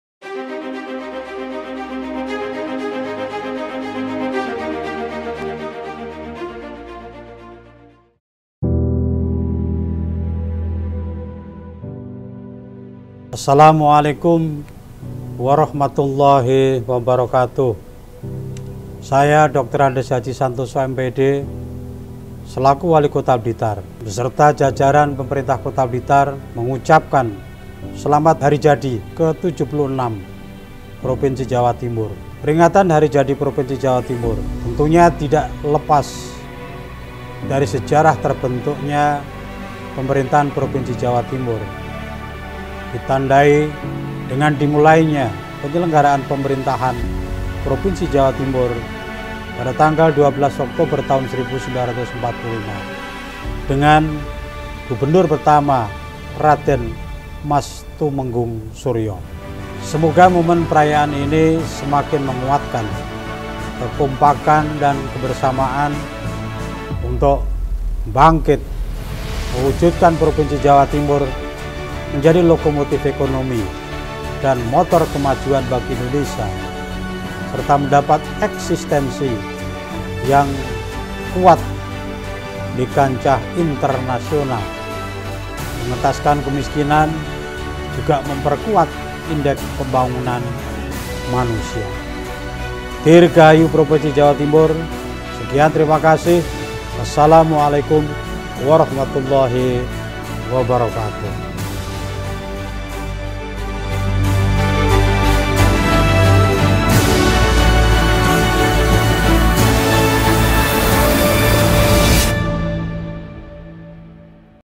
Selamat Hari Jadi Ke - 76 Provinsi Jawa Timur | Wali Kota Blitar
Pemerintah dan Masyarakat Kota Blitar mengucapkan Selamat Hari Jadi ke 76 Provinsi Jawa Timur !

Peringatan Hari Jadi Jawa Timur, tentu saja tidak lepas dari sejarah penyelenggaraan Pemerintah Provinsi Jawa Timur untuk pertama kalinya pada 12 Oktober 1945. Semoga momen perayaan ini semakin memperkuat kekompakan dan kebersamaan untuk bangkit mewujudkan Jawa Timur sebagai lokomatif ekonomi, motor kemajuan bagi Indonesia, mendapat eksistensi yang kuat di kancah Internasional, mengentaskan kemiskinan memperkuat indeks pembangunan kemanusiaan

Aamiin Yaa Robbal alamin